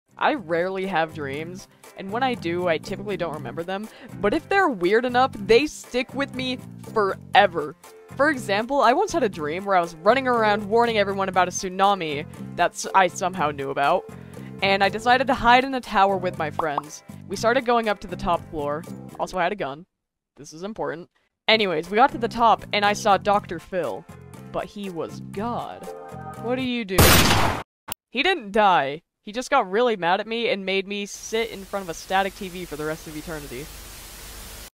My dreams are weird man…
I have very strange dreams. I don’t know why so please don’t ask. I cant explain it